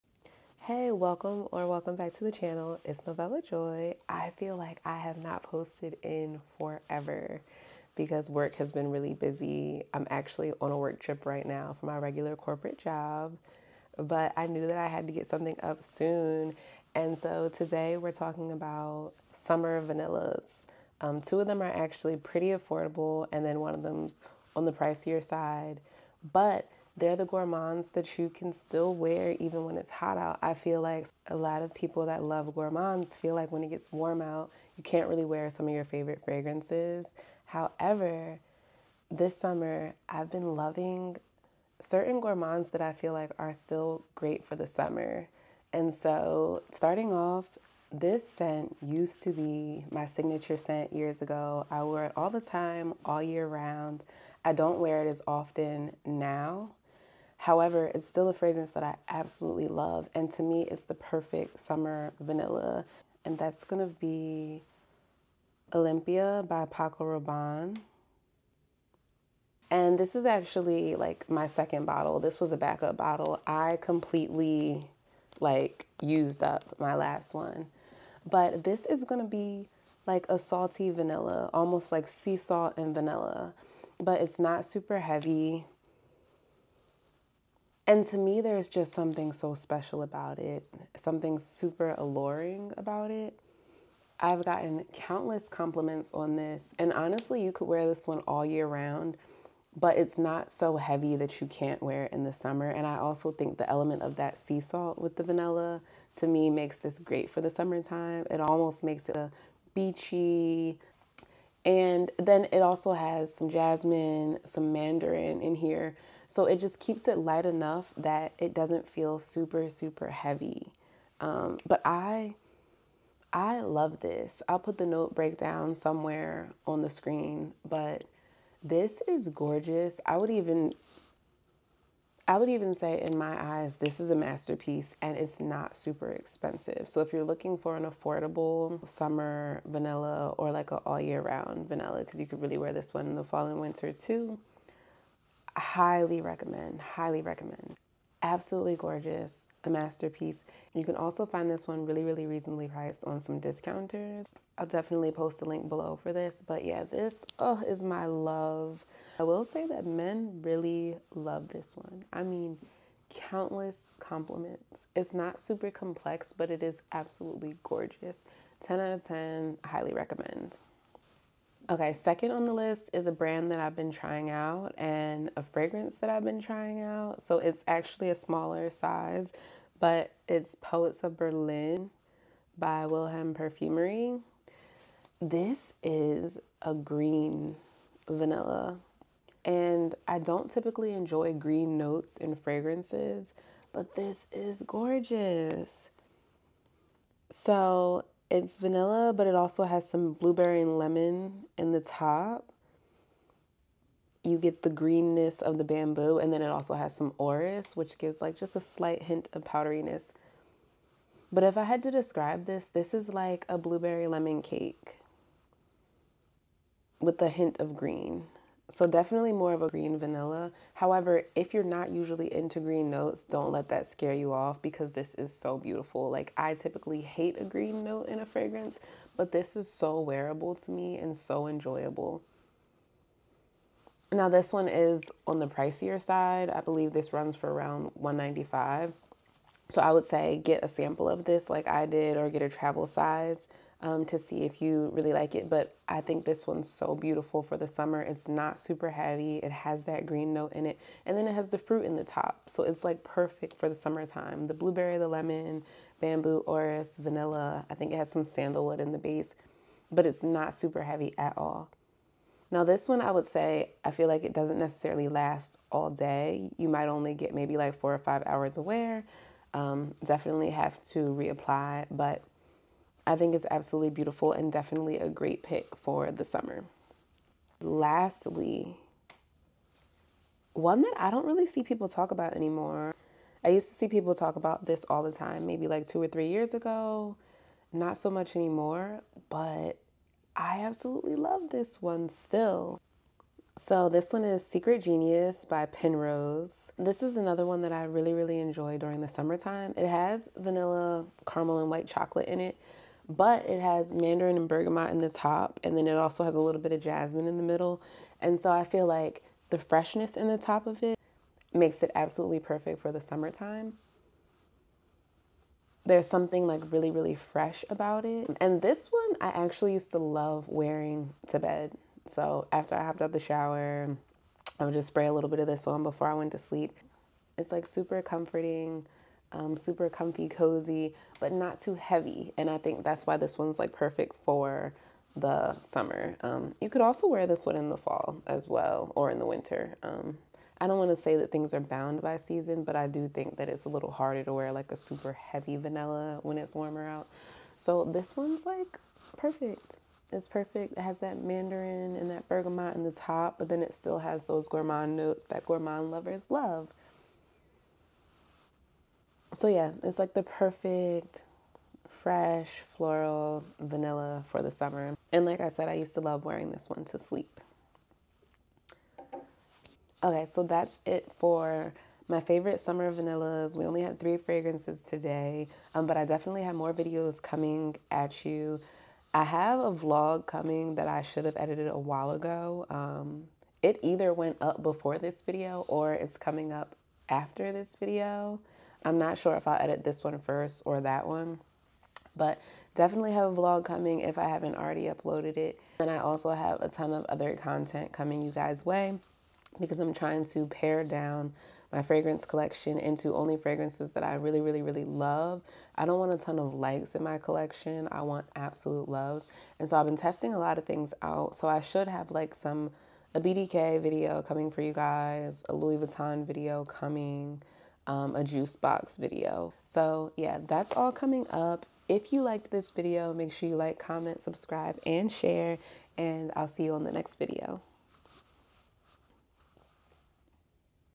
3 Vanilla Fragrances PERFECT For Summer
3 of my favorite vanilla fragrances that I LOVE wearing during the summer! Let me know if you have tried any of these fragrances and if you enjoy them as much as I do 💕.

Pinrose Secret Genius

Timestamps:
Intro 00:00
Paco Rabanne Olympea: 01:05
Vilhelm Perfumerie Poets of Berlin: 03:00
Pinrose Secret Genius: 04:40